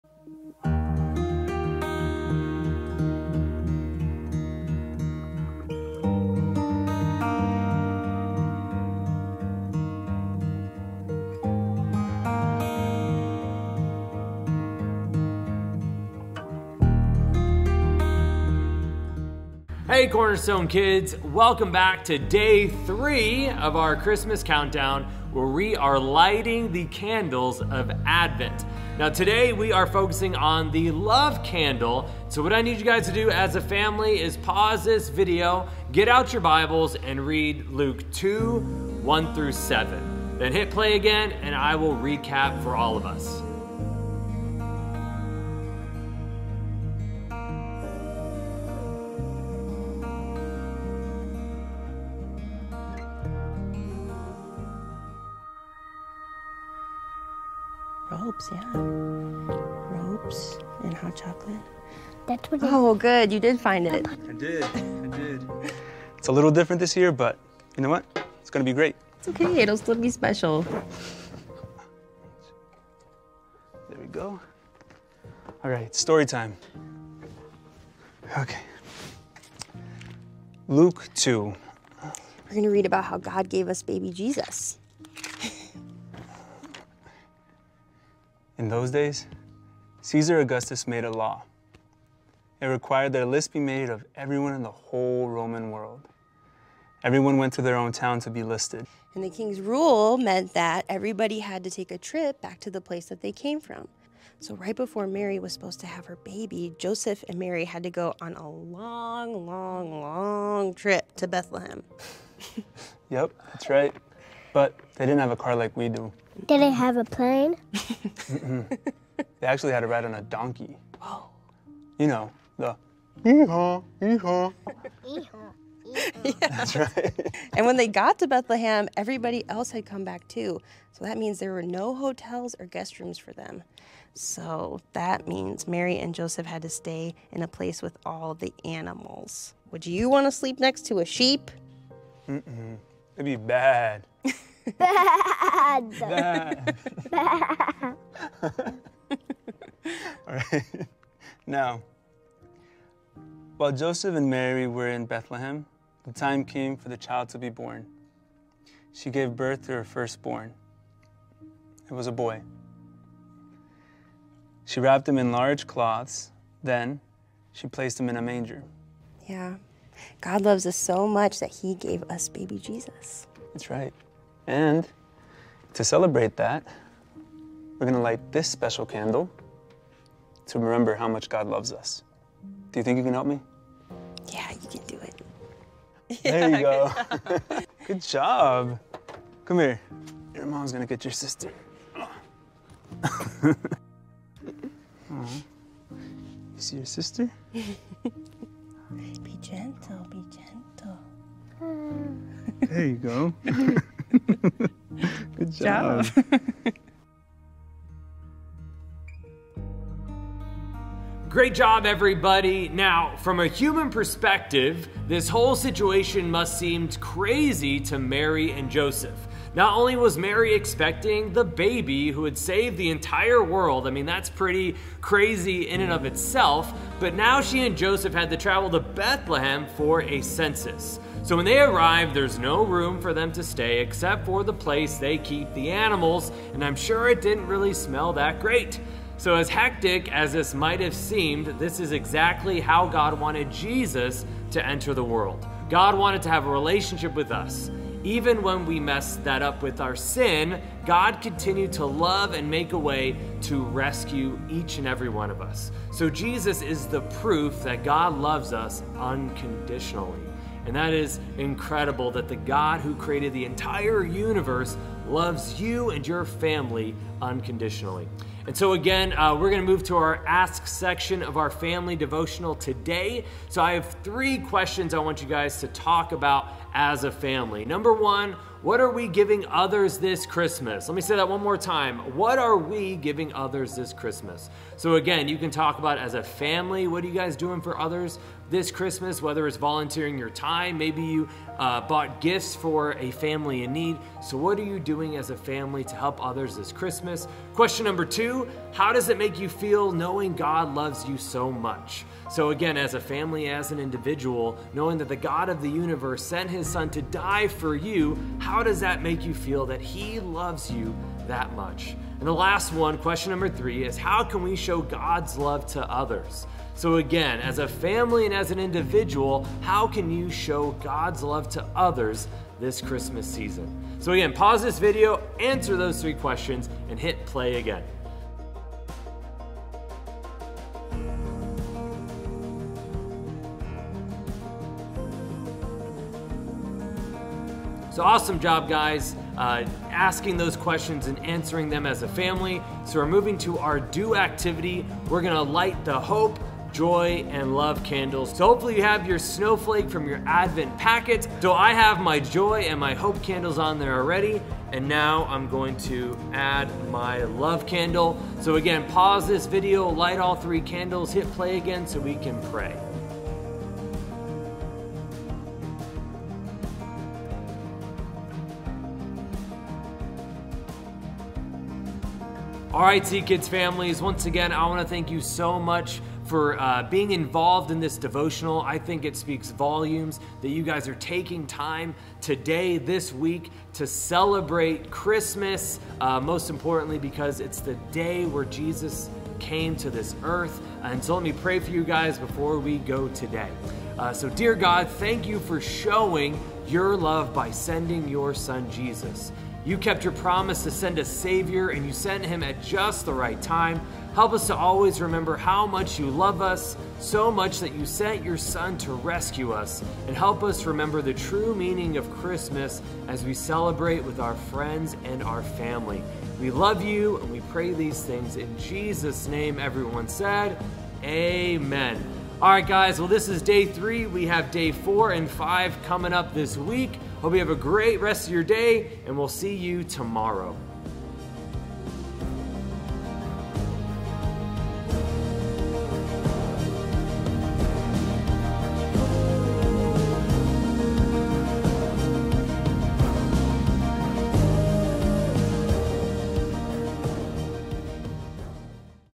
Advent Devo Day 3 - LOVE with Cornerstone Kids
Lighting the candles of Advent is a centuries-old tradition throughout the church. It’s a chance to reflect on significance of the season and look forward to celebrating Jesus. Here’s a simple way to create your own advent tradition. Over the next week, take time each day and countdown to Christmas. Pause and remember the Christmas story. Celebrate how God sent Jesus to be our Savior.

Today we light a candle for LOVE.